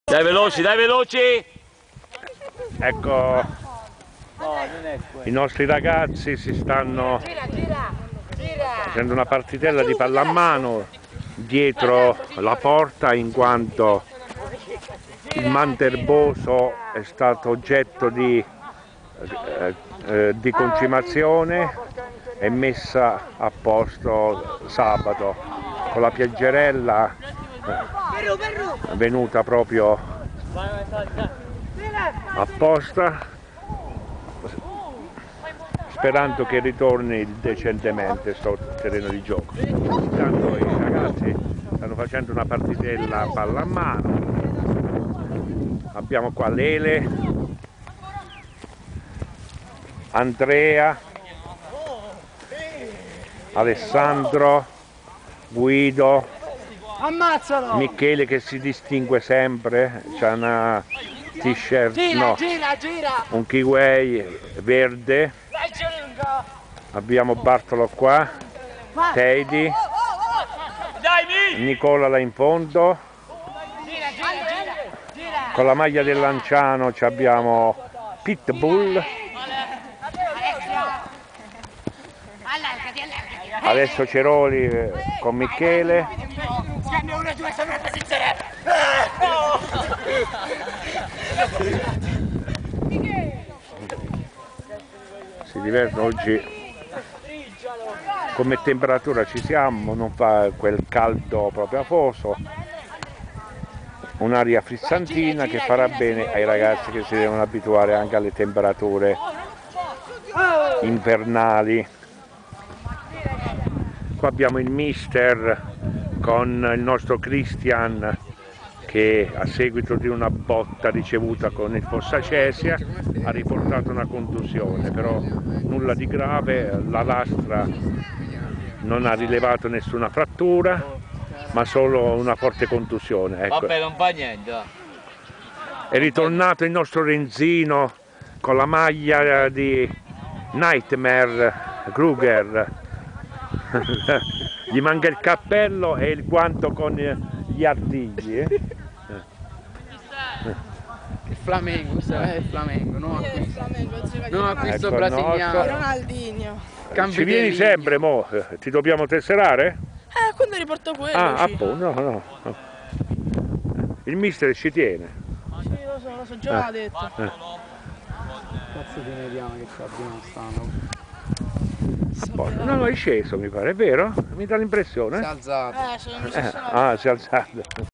VIRTUS CASTEL FRENTANO " ALLIEVI " Stag. 2017-2018
Ieri , Lunedì 11 Settembre 2017 , ripresa degli allenamenti dei ragazzi di Mister Gabriele DI FELICE . Considerato che il  manto erboso " Nuovo "  di Via Olimpia era inibito in quanto oggetto di concimazione e lavori , i ragazzi si sono allenati dietro le porte .Da oggi gli allenamenti si svolgeranno al Campo Sportivo " Della Vittoria " di Via Nazionale , così come i ragazzi Giovanissimi di Mister Giovanni DI GIULIO .